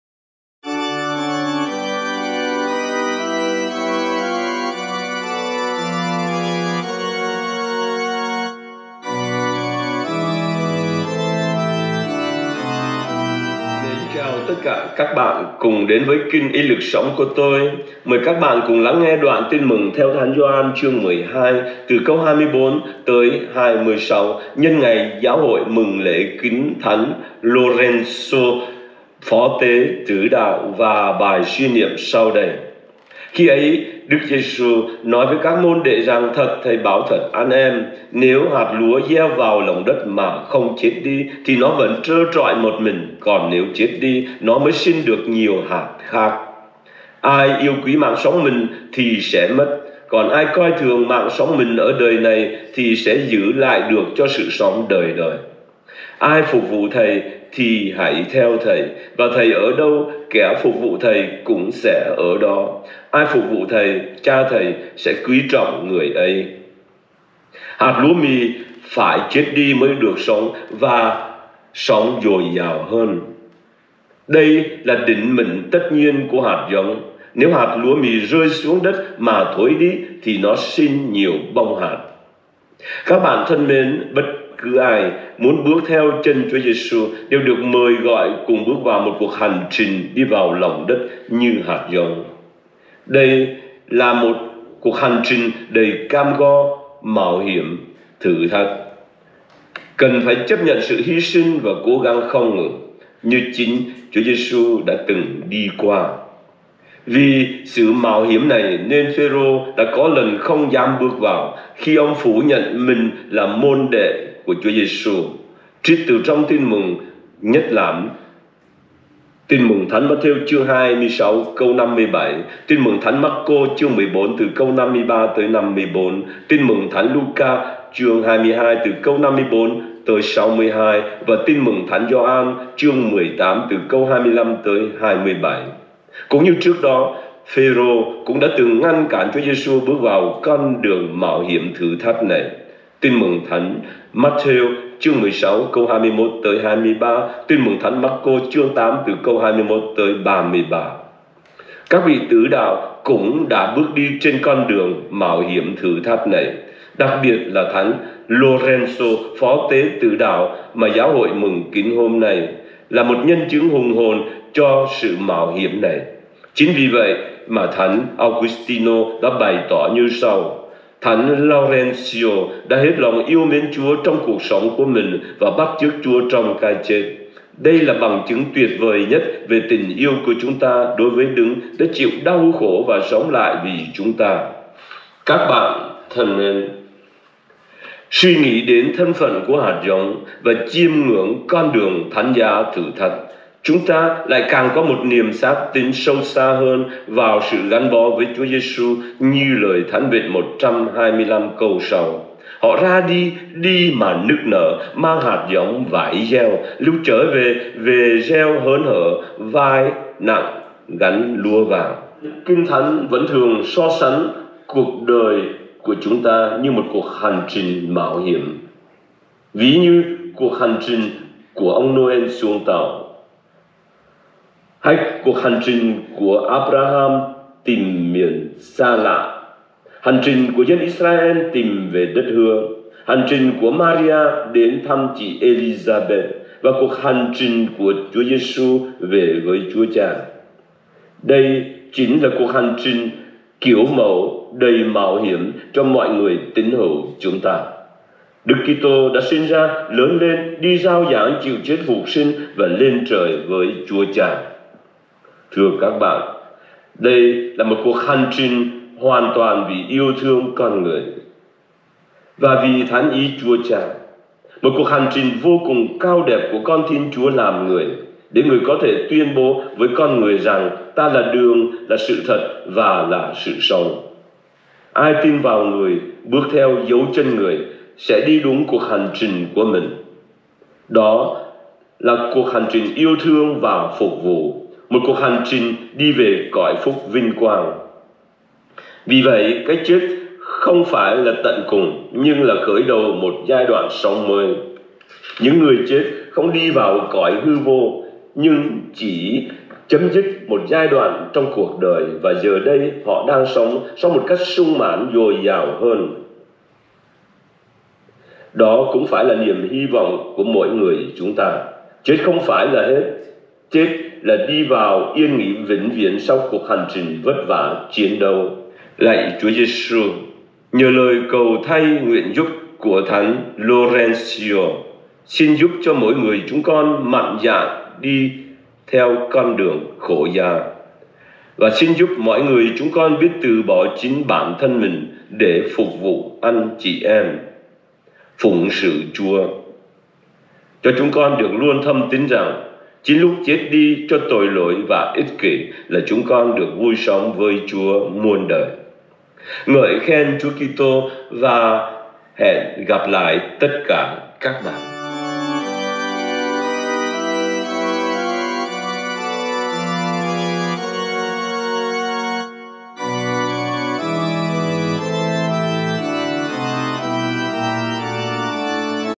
Suy niệm lời Chúa mỗi ngày Ga 12, 24-26. Ngày 10.8 Lễ kính thánh Laurensô phó tế tử đạo.
Số mệnh của hạt giống.